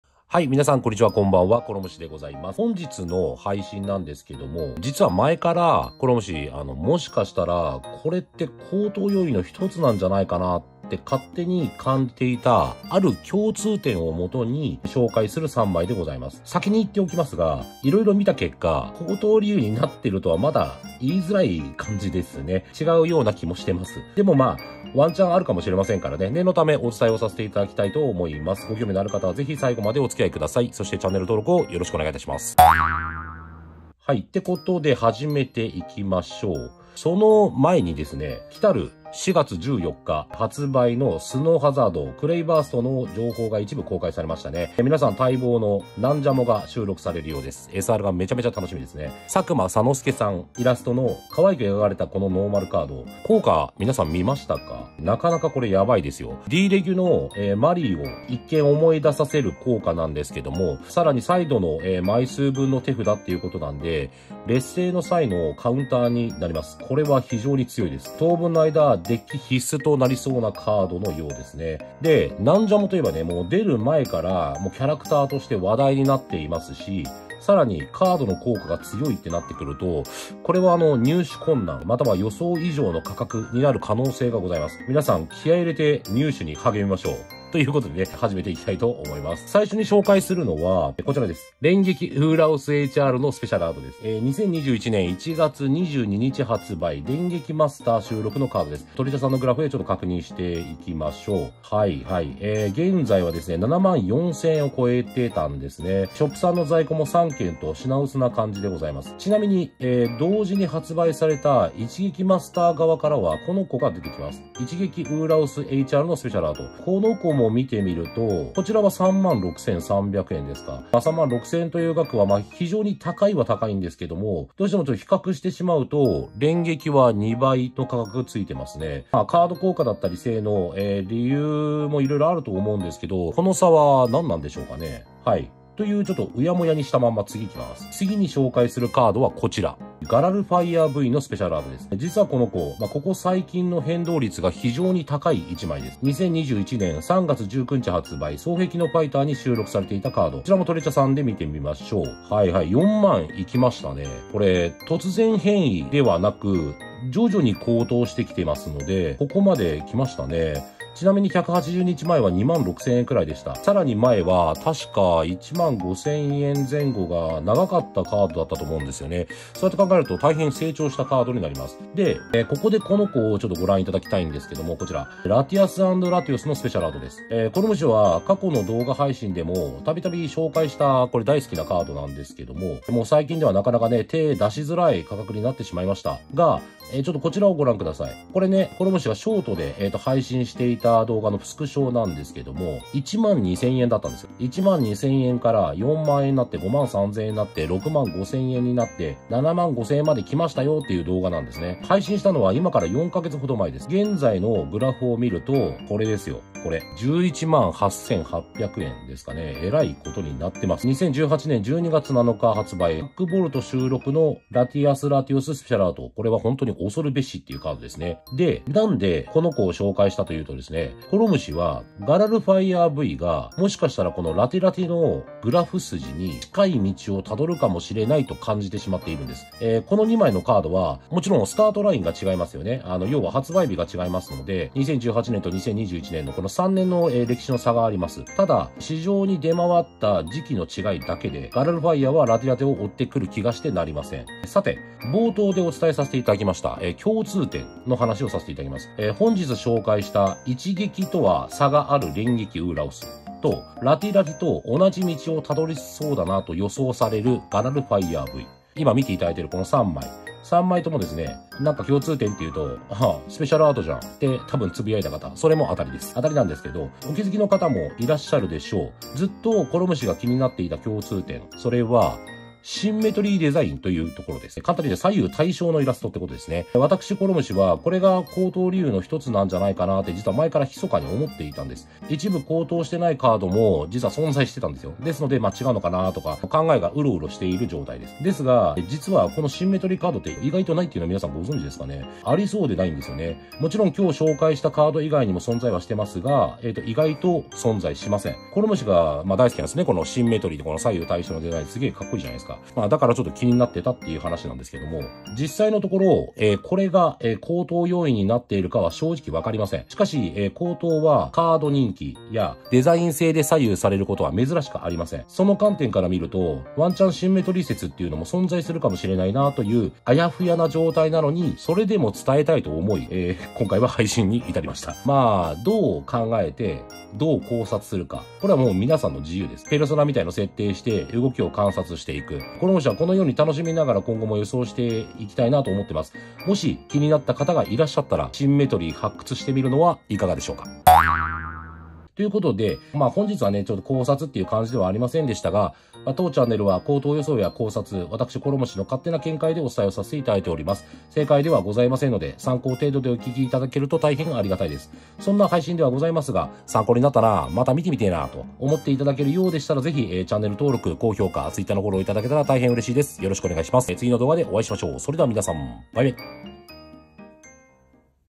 【ポケカ高騰】共通点を探し高騰を考察せよ！高騰要因は色々な角度から探るべき！あくまでcoromushiの見解 れんげきウーラオス ラティラティ ガラルファイヤー SA スペシャルアート ポケモンカード
【動画内で使用したグラフのサイト トレチャ】

★もう売っちゃう？！ちょっと待った！★